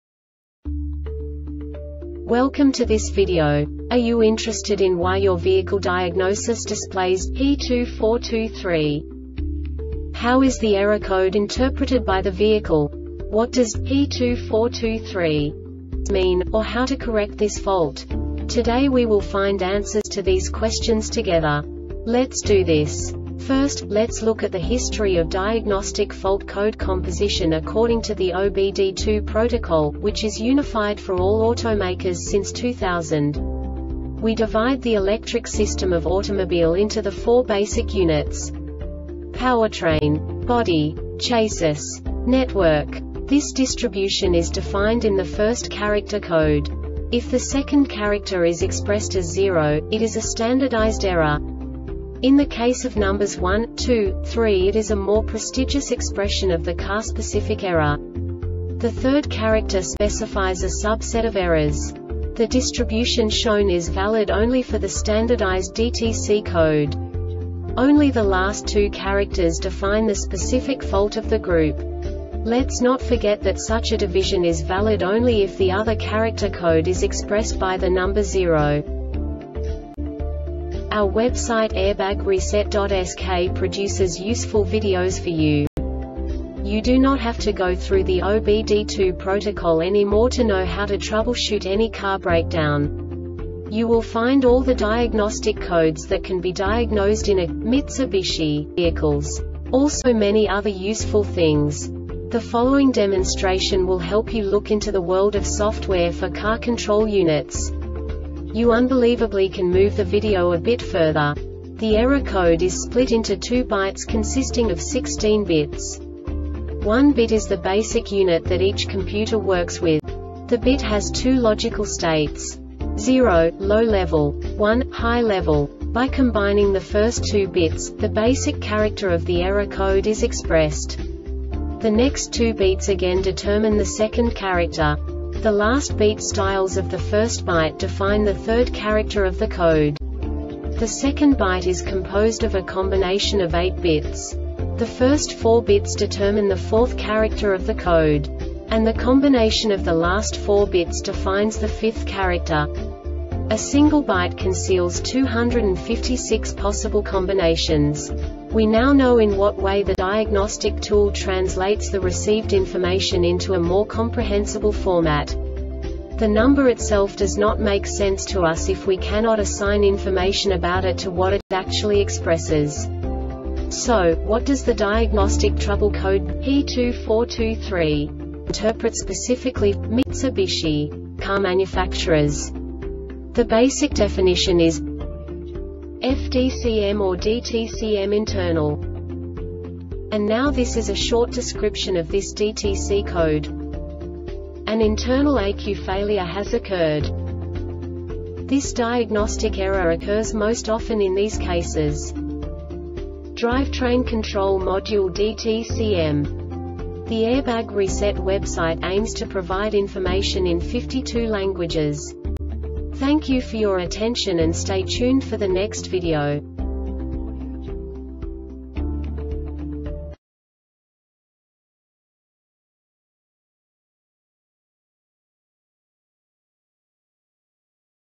DTC Mitsubishi P2423 Short Explanation
Contents:
0:21 Basic DTC analysis according to OBD2 protocol standard.
1:48 Insight into programming
2:58 A diagnostically understandable description of the Diagnostic Trouble Code
3:05 Basic error definition

Make:
Mitsubishi
Code:
P2423
Definition:
FDCM or DTCM Internal
Description:
An internal ECU failure has occurred.
Cause:
Drivetrain Control Module (Dtcm)